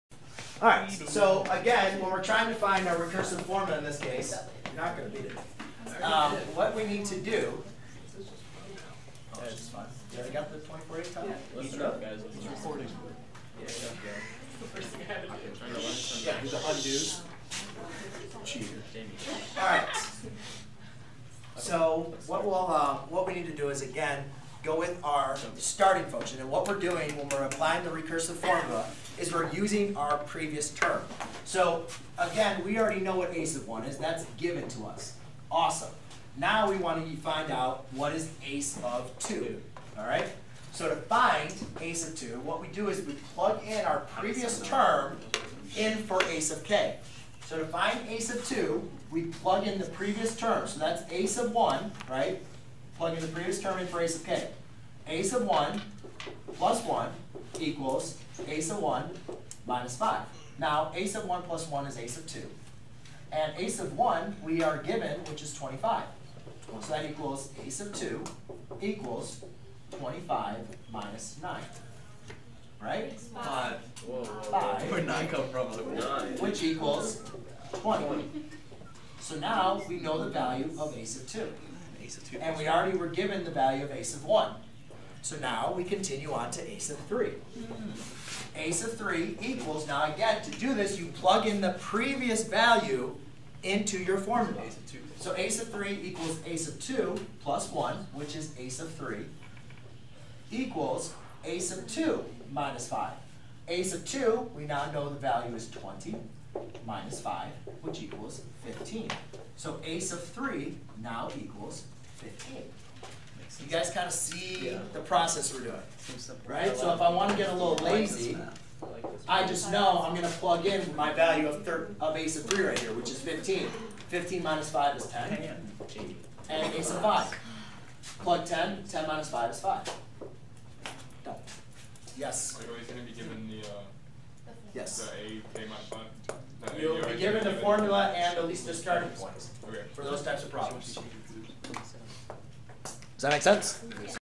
How to use the recursive formula to evaluate the first five terms
👉 Learn all about recursive sequences. Recursive form is a way of expressing sequences apart from the explicit form. In the recursive form of defining sequences, each term of a sequence is expressed in terms of the preceding term unlike in the explicit form where each term is expressed in terms of the first term.

The general form of the recursive arithmetic sequence is given by A_(k+1) = A_k + d, where A_(k+1) is the next term of the sequence, A_k is the current term and d is the common difference. The general form of the recursive geometric sequence is given by A_(k+1) = (A_k)r, where A_(k+1) is the next term of the sequence, A_k is the current term and r is the common ratio.

In the recursive form of sequence, it is usually very important to indicate an initial value for the sequence.